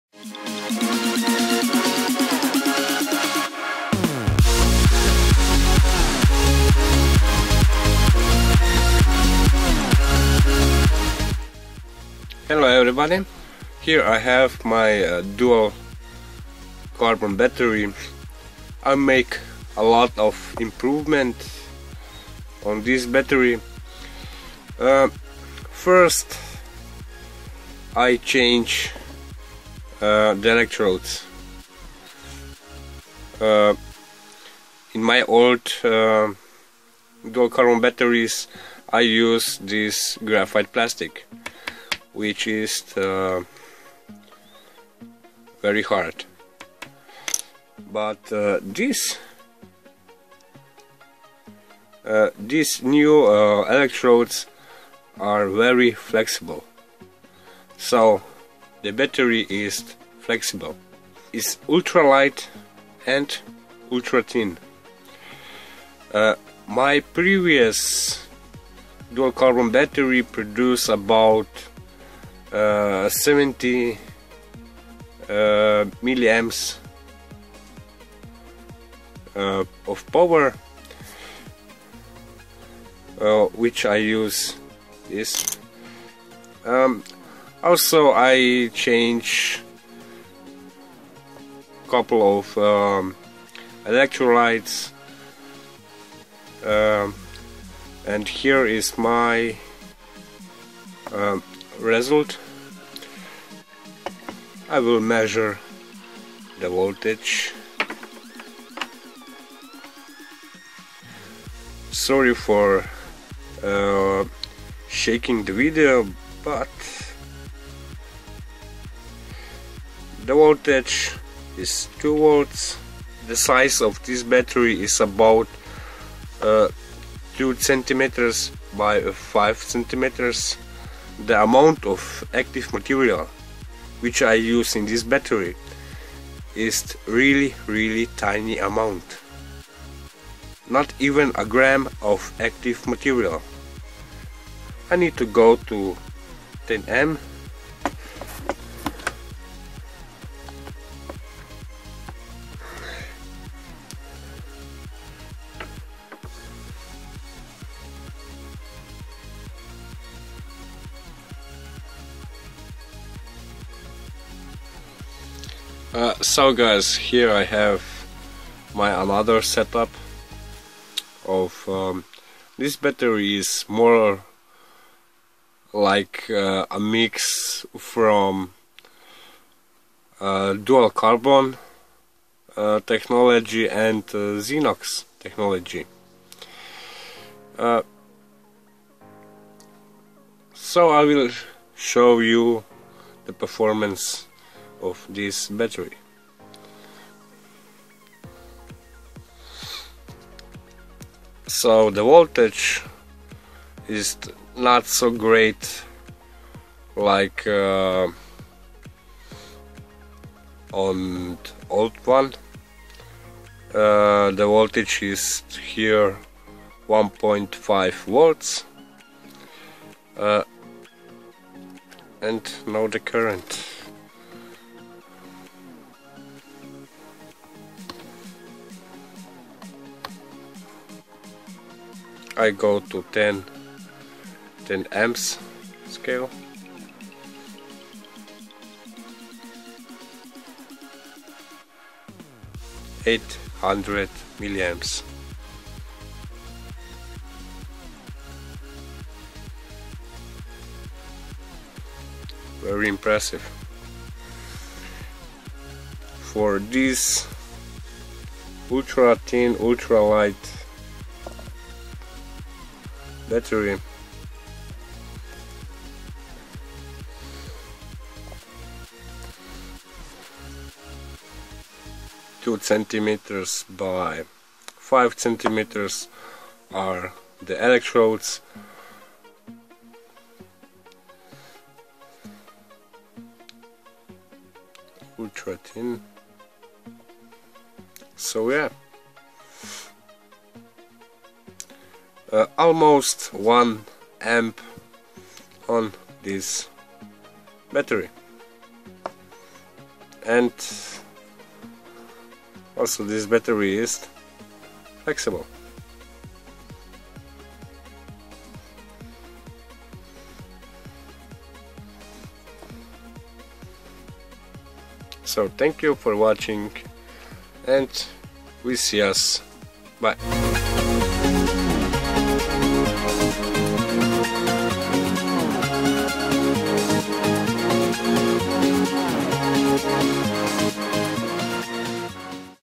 Dual Carbon Battery 2017 Edit
New update on Dual Carbon Rechargeable Battery!
New Dual Carbon Battery Design produce a 800 mA of power with 0.4 g of active material (old dual carbon battery produce 70 mA with same amount of active material).